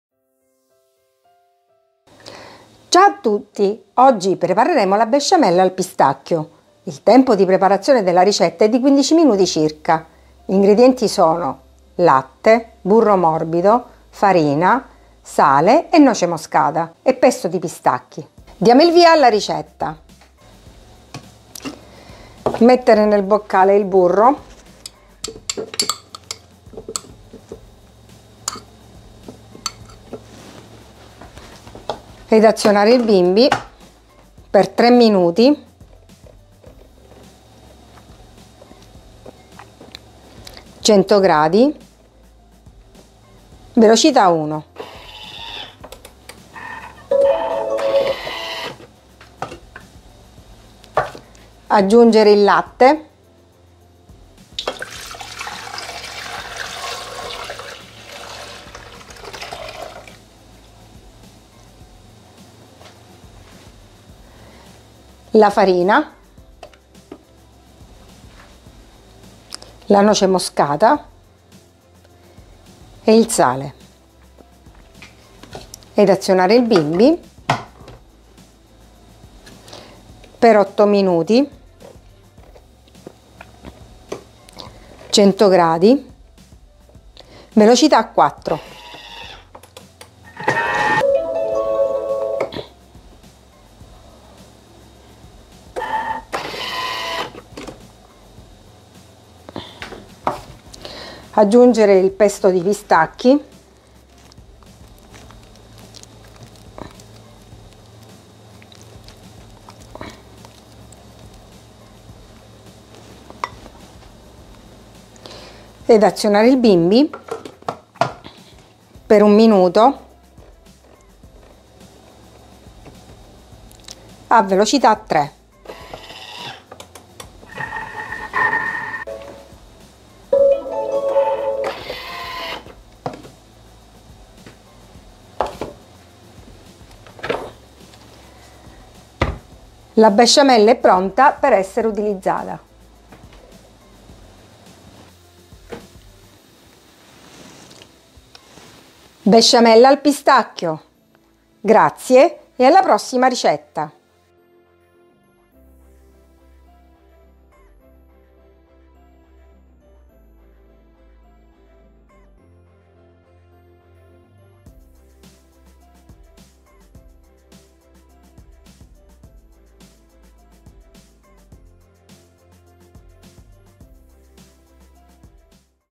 Besciamella al pistacchio per bimby TM6 TM5 TM31 TM21
Dosi per 1,200 litri di besciamella

Ingredienti
1 litro di latte
80 g di burro morbido
80 g di farina
1 cucchiaino di sale
noce moscata q.b.
260 g di pesto di pistacchi

English
ingredients
1 liter of milk
80 g of softened butter
80 g of flour
1 teaspoon of salt
nutmeg to taste
260 g of pistachio pesto
Quantities for 1,200 liters of béchamel sauce

Español
ingredientes
1 litro de leche
80 g de mantequilla suave
80 g de harina
1 cucharadita de sal
nuez moscada al gusto
260 g de pesto de pistachos
Proporciones para 1.200 litros de bechamel

Deutsch
zutaten
1 Liter Milch
80 g weiche Butter
80 g Mehl
1 Teelöffel Salz
Muskatnuss nach Geschmack
260 g Pistazienpesto
Mengenangaben für 1.200 Liter Bechamelsauce